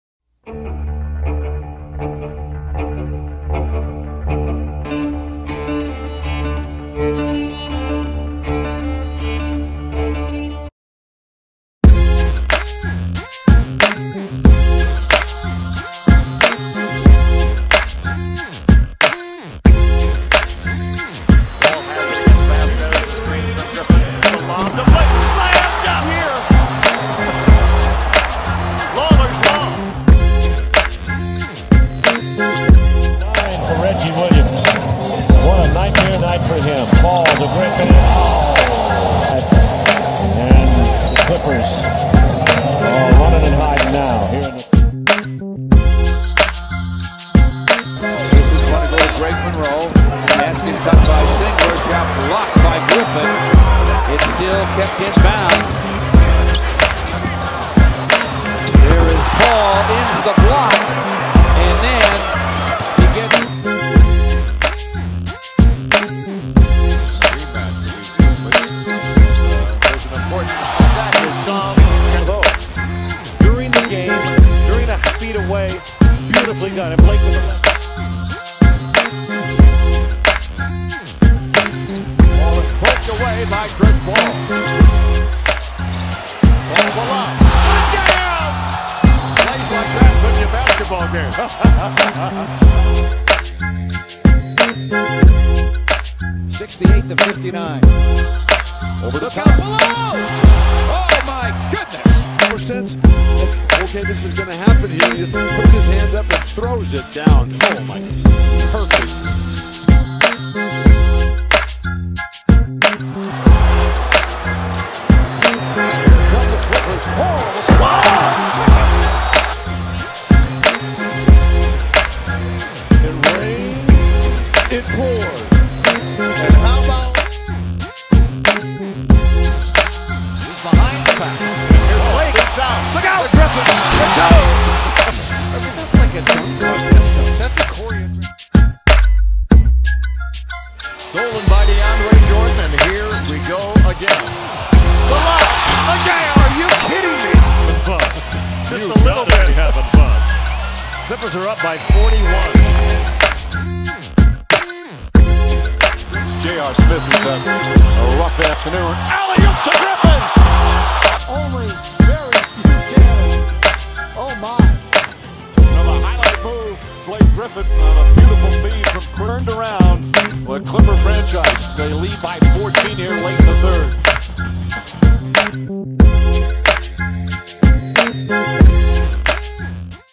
Top 10 Chris Paul - Blake Griffin Connections
Top 10 Plays From Chris Paul and Blake Griffin Plays Los Angeles Clippers.